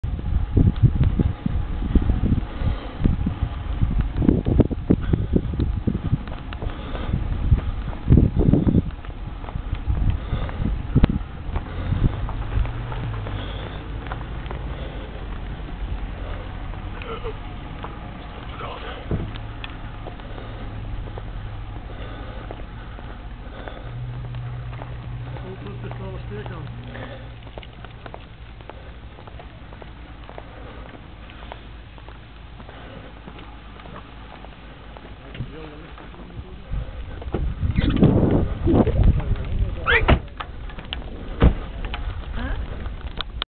Aankomst per racefiets op Col d'Allos
Aankomst per racefiets van Raf op de Col d'Allos vanaf Barcelonnette op 9 september 2010.
Clubreis Nissorap Barcelonnette september 2010